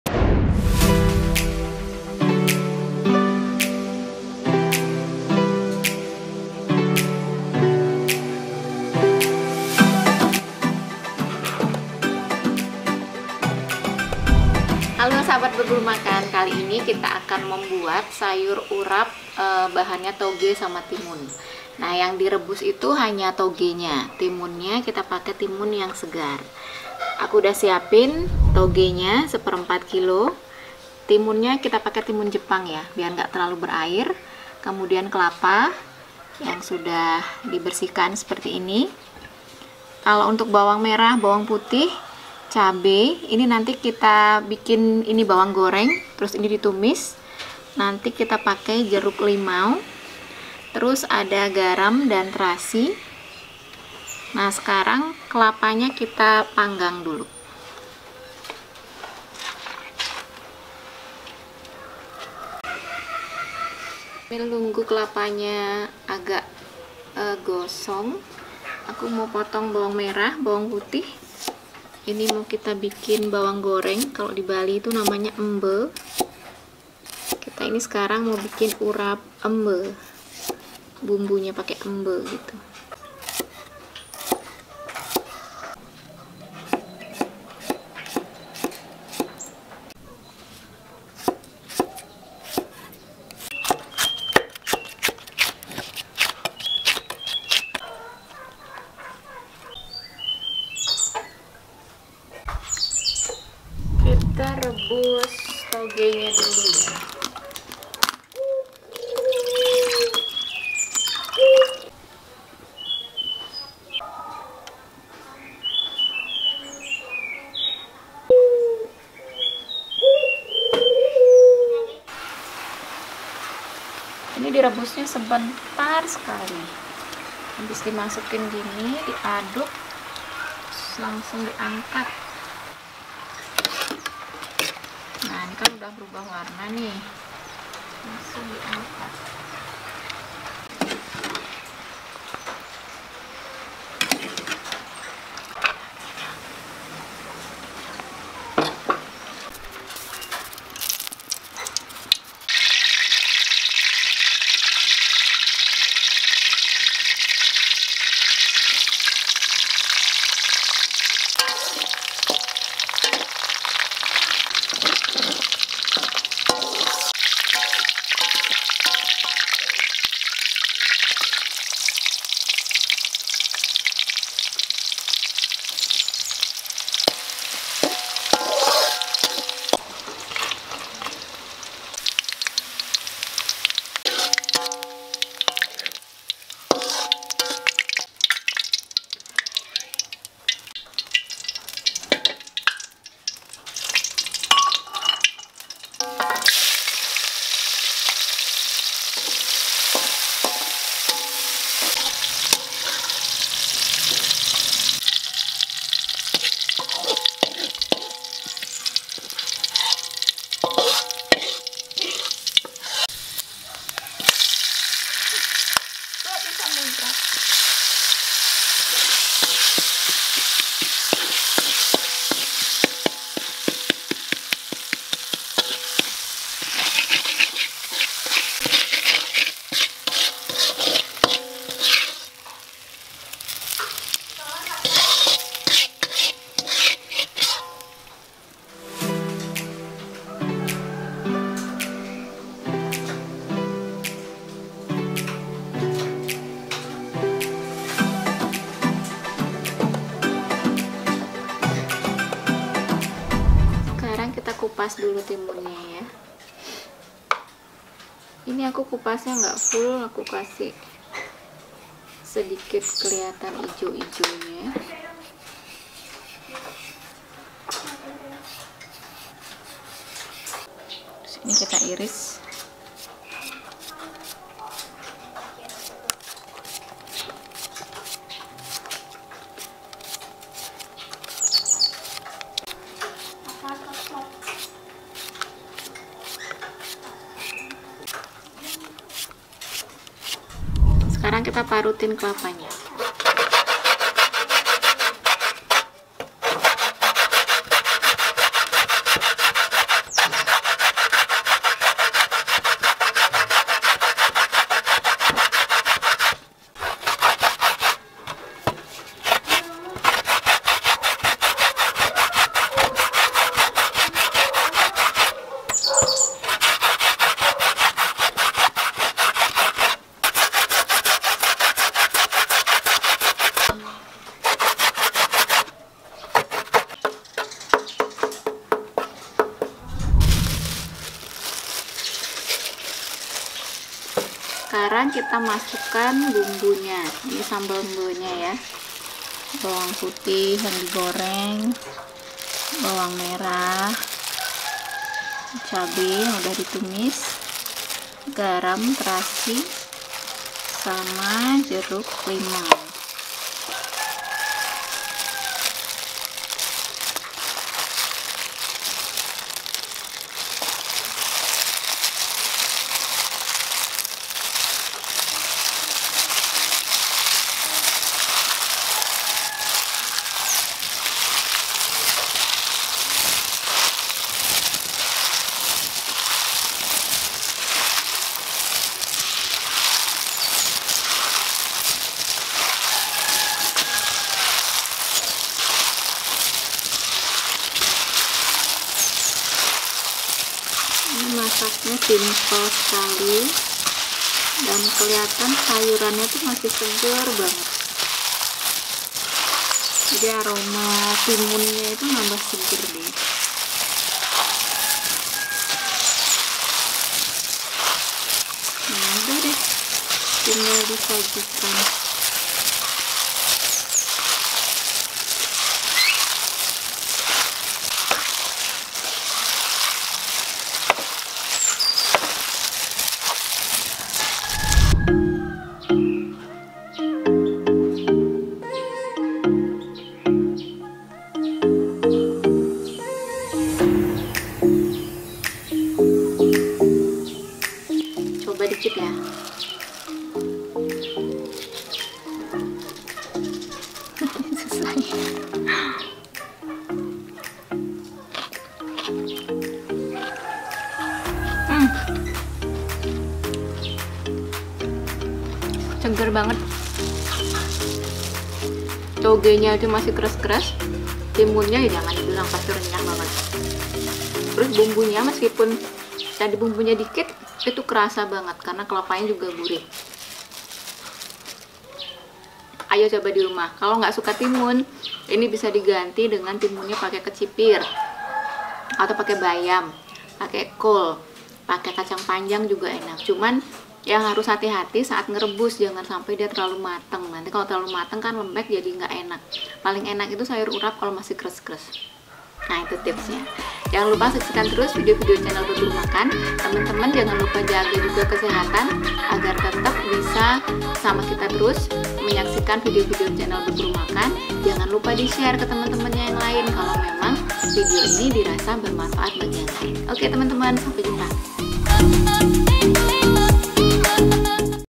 RESEP CARA MEMBUAT SAYUR URAP TOGE TIMUN YANG SUPER KRENYEZZZ
Kali ini Team Berburu Makan akan bereksperimen lagi membuat resep Resep Sayur Urap Toge Timun yang super Krenyes. Sayur urap ini sangat enak disajikan bersama nasi hangat di sore hari yang cerah. Nah penasaran enaknya sobat, tonton videonya ya!
Cara Pembuatan silahkan di tonton videonya sampai habis ya sobat!
FP : Berburu Makan
Under Copyright @Berburu Makan Youtube Channel
Music in this video!